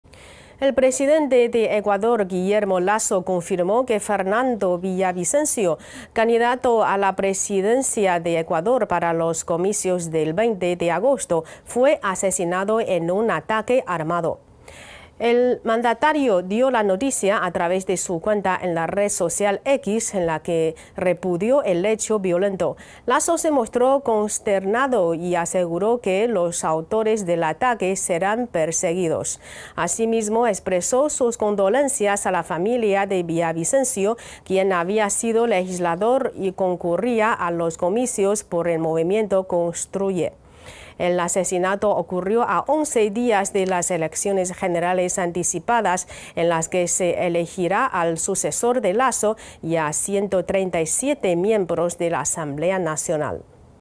Asesinan a Fernando Villavicencio, candidato a la presidencia de Ecuador
El presidente de Ecuador, Guillermo Lasso, confirmó que Fernando Villavicencio, candidato a la presidencia de Ecuador para los comicios del 20 de agosto, fue asesinado en un ataque armado. El mandatario dio la noticia a través de su cuenta en la red social X, en la que repudió el hecho violento. Lasso se mostró consternado y aseguró que los autores del ataque serán perseguidos. Asimismo, expresó sus condolencias a la familia de Villavicencio, quien había sido legislador y concurría a los comicios por el Movimiento Construye. El asesinato ocurrió a 11 días de las elecciones generales anticipadas, en las que se elegirá al sucesor de Lasso y a 137 miembros de la Asamblea Nacional.